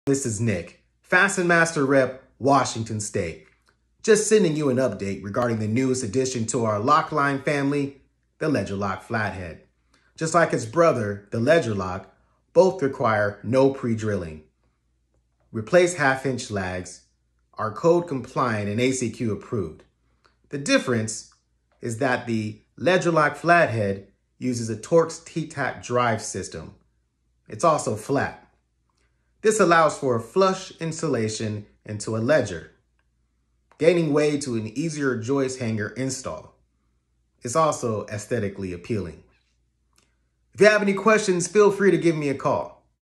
LedgerLOK Flathead
The newest addition to our LOKline family. The LedgerLOK Flathead. It has all the benefits of its brother “the LedgerLOK” but one difference. It’s flat ! Tnis will allow joist hanger to be installed without anything in the way.